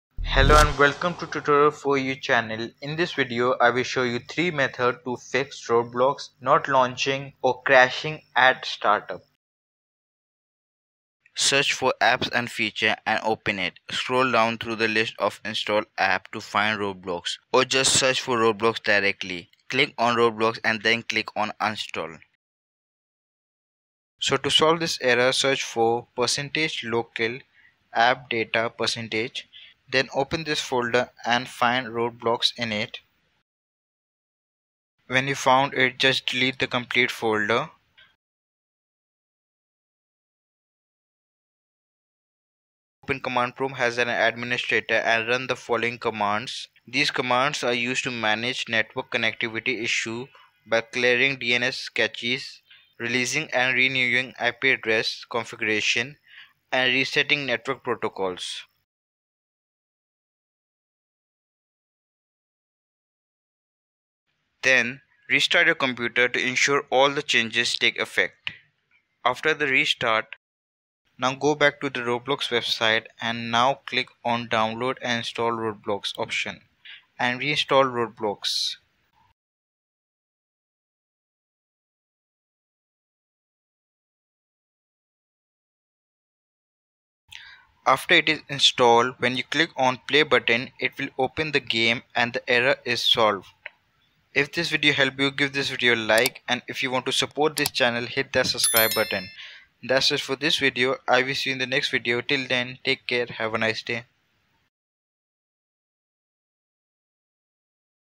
How To Fix Roblox Not Launching (2024)
Are you facing issues with Roblox not launching on your PC? Don't worry; we've got you covered! In this comprehensive guide, we'll walk you through the steps to resolve common problems related to Roblox not launching, including Roblox Player not launching.

🔧 In this video, we'll cover:

Common reasons why Roblox may not be launching on your PC.
Step-by-step instructions to diagnose and fix these issues.
Troubleshooting tips for both Roblox and Roblox Player.
Additional solutions to ensure a smooth Roblox gaming experience.
Whether you're a seasoned Roblox player or new to the game, our tutorial will help you get back into the action in no time. Don't let technical glitches keep you from enjoying your favorite Roblox games.

Have a specific issue you'd like us to address? Drop it in the comments, and we'll do our best to assist you!